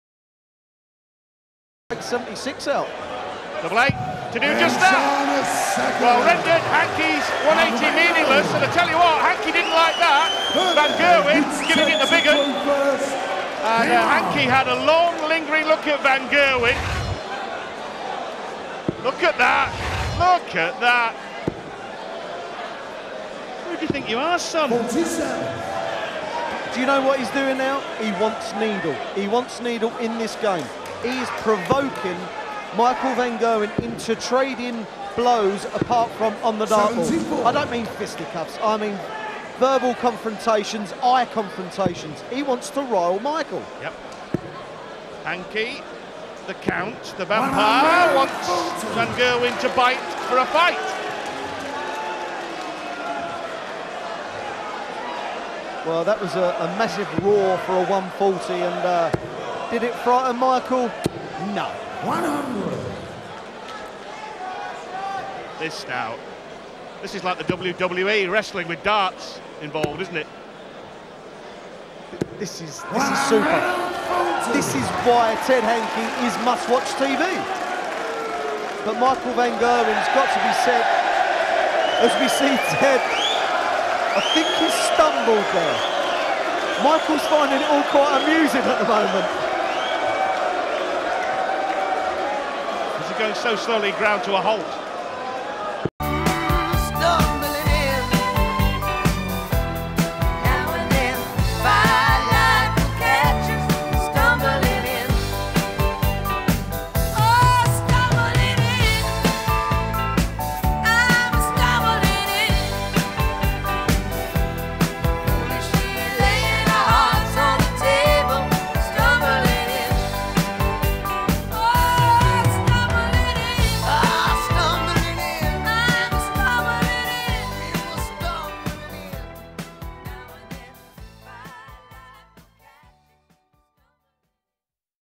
Ted Hankey Stumbling in
Ted Hankey stumbles on stage 2011 grand slam darts